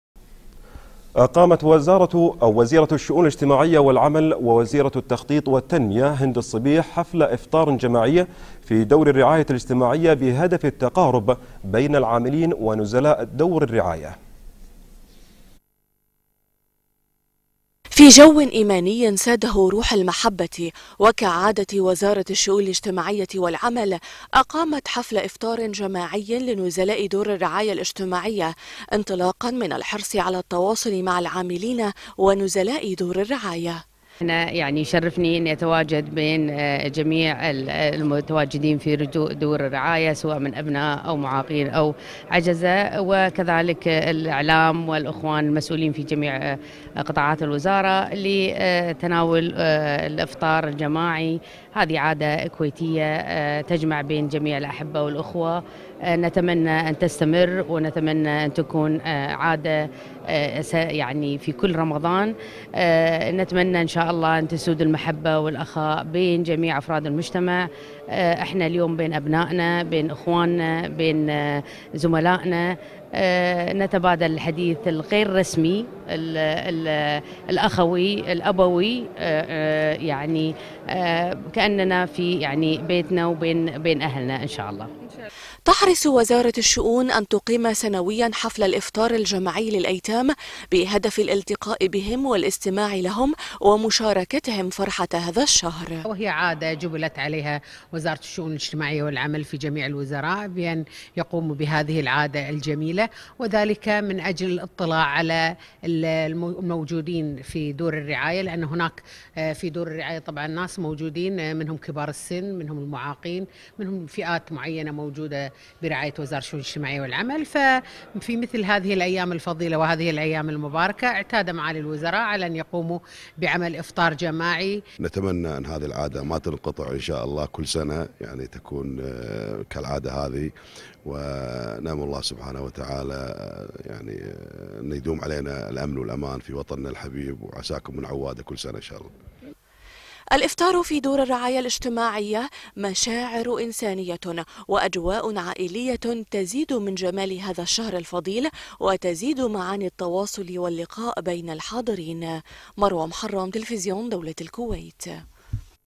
وزارة الشؤون الاجتماعية حفل افطار بدور الرعاية
تلفزيون،الكويت،وزارة الشؤون الاجتماعية والعمل تقيم حفل افطار بدور الرعاية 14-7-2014